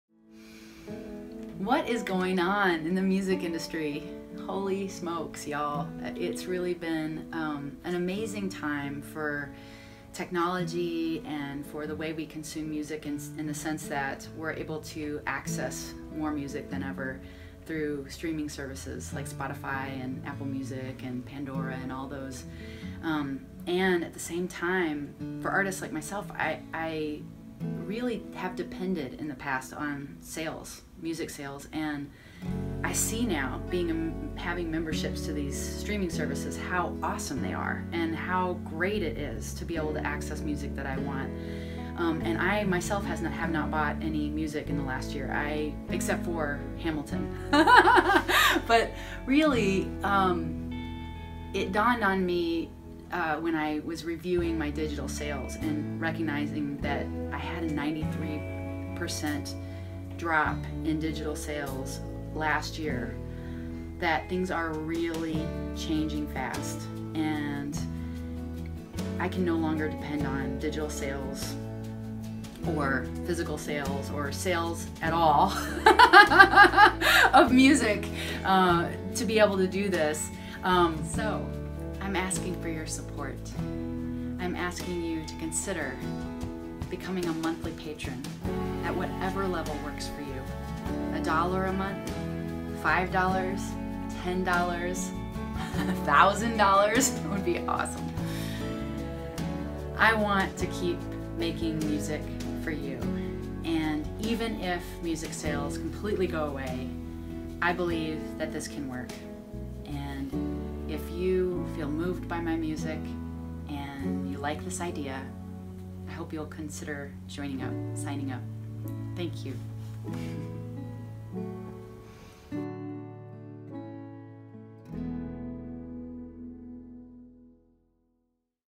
Ellis' 2 min take on the music business and patronage
Ellis sums up the music business and fundraising in about 2 minutes.